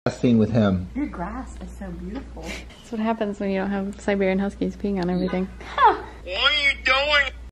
SML Drama: AUDREY CALLS OUT CHILLY’s DOGS
:O First time Audrey said anything about chilly and her huskies